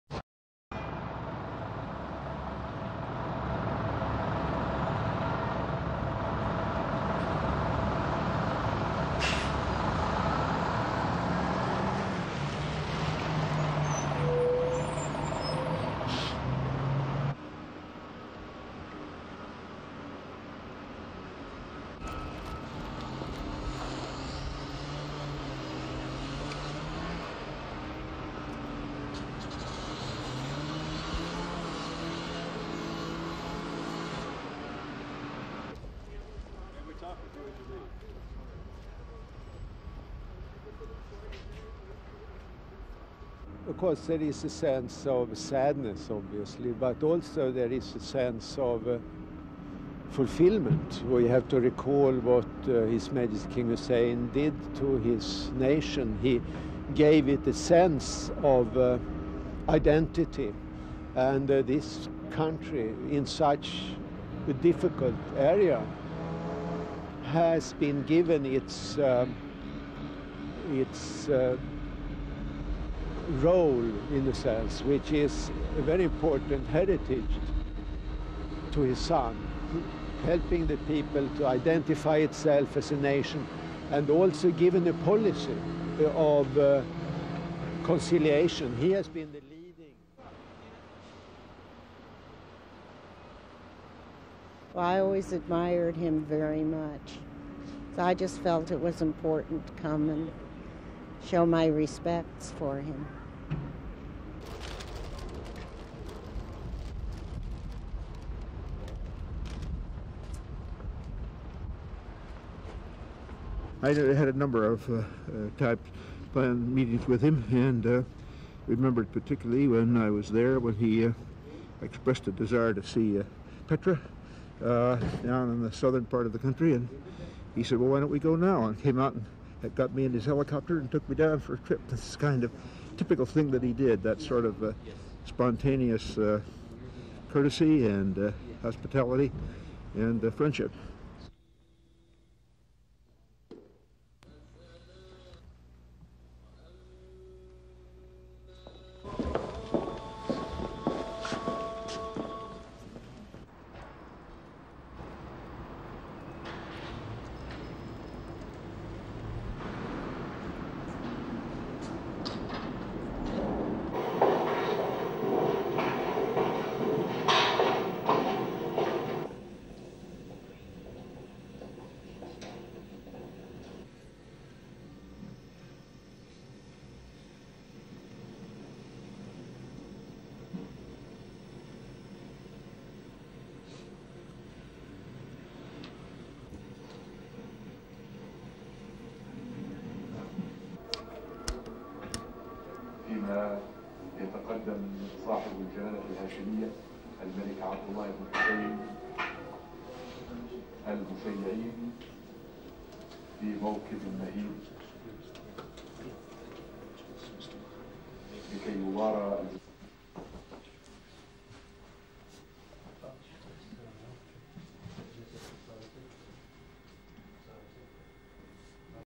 US/UK: WORLD PAYS TRIBUTE TO KING HUSSEIN
People across the world are paying their respects to King Hussein.

Mourners in places as far apart as Washington and London filed into the Jordanian  embassies to lay flowers and sign a book of condolences.

They remembered the King as a symbol of endurance and a voice for peace in a  landscape of crisis and war.

In New York, the United Nations lowered its flag to halfmast in respect for the dead King  of Jordan.

And across the street from the U-N, the U-S mission also paid tribute to the former  statesman and king.

In his 46 years on the throne, King Hussein nurtured close ties with the United States.

And in Washington on Monday, those who were touched in some way by the king paid  their respects.

Former U-N inspector Rolf Ekeus said Hussein elevated the stature of his desert  Kingdom and this would forever be his legacy.

SOUNDBITE: (English)
"Of course there is a sense of sadness but also there is a sense of fulfillment. We have  to recall what his majesty King Hussein did to his nation, he gave it a sense of identity  and this country in such a difficult area has been given  its role in a sense which is a  very important heritage to his son, helping the people identify itself as a nation and  given the policy of conciliation."
SUPER CAPTION: Rolf Ekeus, Swedish Ambassador to the United States

Others came to the Jordanian embassy out of a sense of duty.

SOUNDBITE: (English)
"I always admired him very  much. I just felt it was important to come and pay my  respects for him."
SUPER CAPTION: Helen Kirkpatrick, Voxpop

In his long rule of Jordan, King Hussein dealt with nine U-S presidents.

Former Secretary of State Casper Weinberger remembered the King as a gracious and  fun-loving host.

SOUNDBITE: (English)
"I had a number of meetings with him and I remember particularly when I was there, I  expressed a desire to see Petra, in the southern part of the country and he came out  and got me in his helicopter and took me down for a trip. This is typical of what he did,  that sort of spontaneous courtesy and hospitality and friendship."
SUPER CAPTION: Casper Weinberger, former Secretary of State in the Reagan  administration

The respectful continued to file into the embassy and signed their names in the book of  condolences.

Around the world, people from all walks of life paid their respects and prayed for the  deceased King.

The Jordanian embassy in the United Kingdom flew its flag at halfmast.

Officials gathered inside the embassy to watch king Hussein's funeral broadcast live on  television.

Royals, presidents, prime ministers and officials from about 40 countries attended the  King's funeral, bringing adversaries from across the globe together.